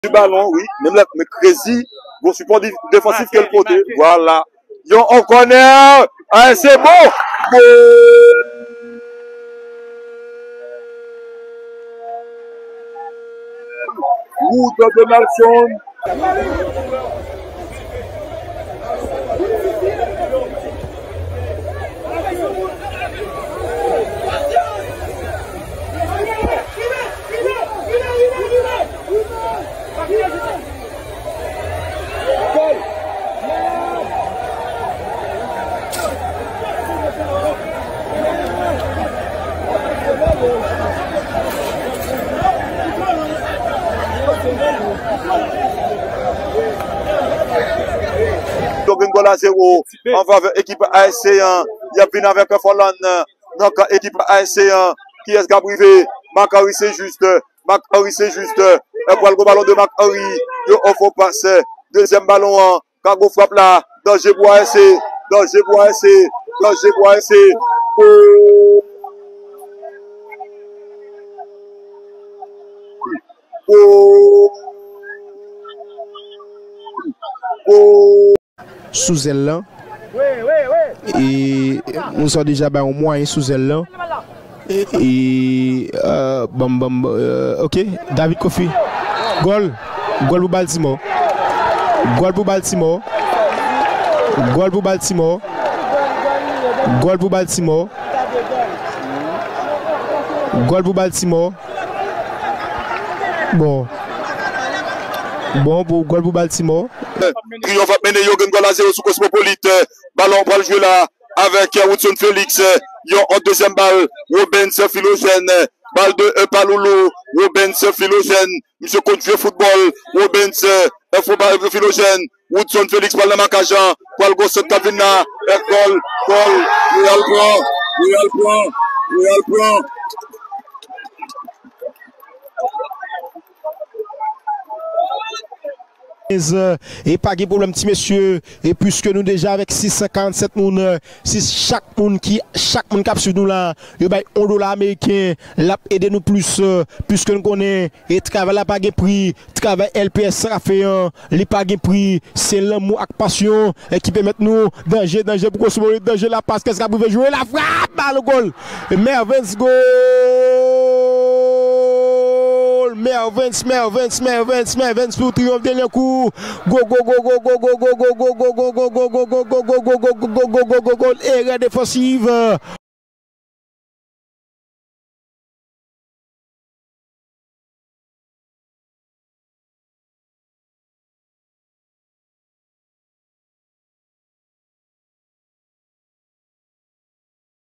Tout gòl setyèm jounen Chanpyona Nasyonal D1 Spécial la | Serie d’ouverture 2024
Crédit
LPS media groupe
Actualité dans le monde
Cap news

Buteurs
Mervens Stamar (Triomphe)
Fredelin Philistin (Ouanaminthe)
Wood Donaldson (AS Capoise)
Michel Anio (Racing FC)